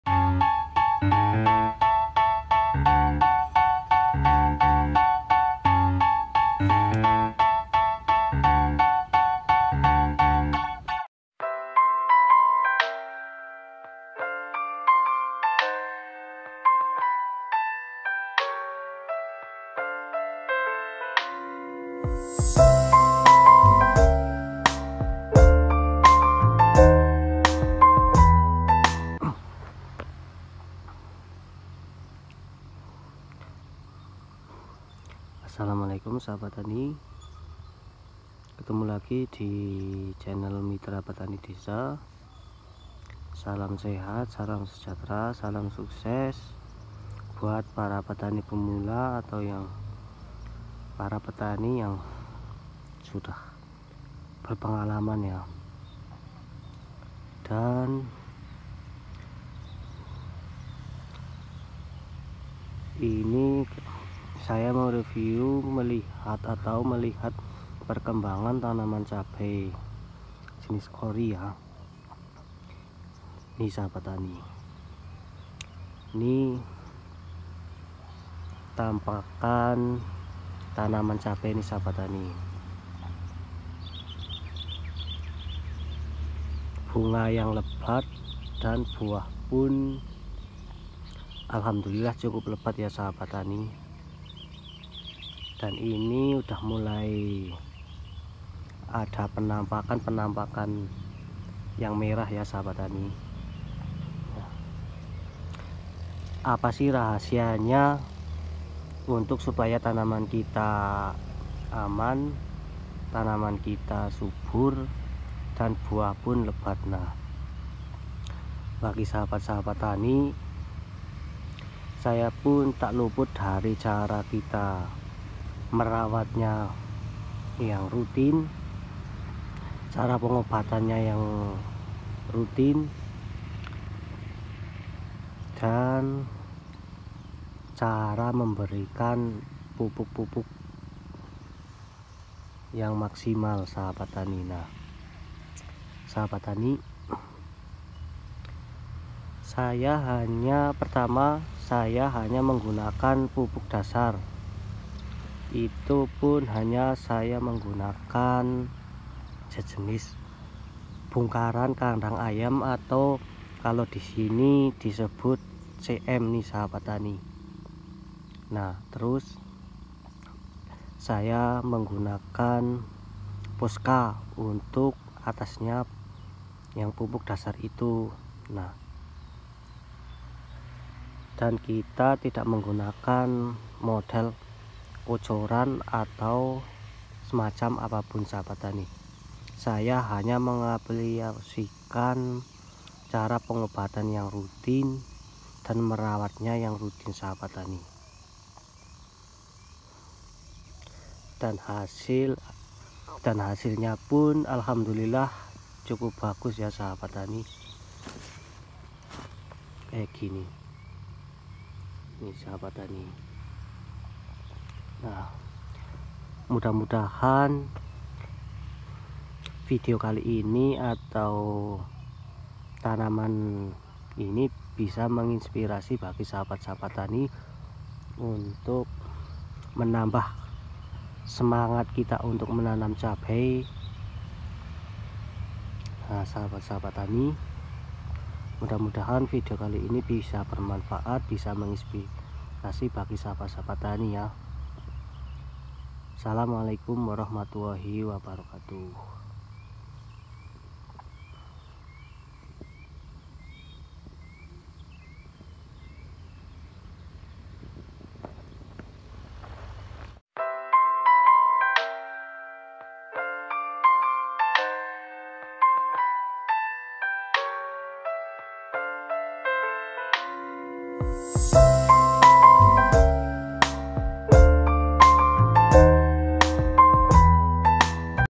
PETANI CABE WAJIB MENCOBA??! LEBAT,SEHAT & AMPUH DARI SERANGAN PENYAKIT
Semoga bermanfaat bagi para petani pemula, sangat merekomendasikan tanaman cabe, cara perawat dan cara penyemprotan semoga berguna , dan minta maaf bahasa dari kata kata saya masih belepotan🙏🙏🙏🙏 salam petani desa.                                                            Tag; tanaman cabe,
efek asam amino pada tanaman cabe. Hastag;caberawit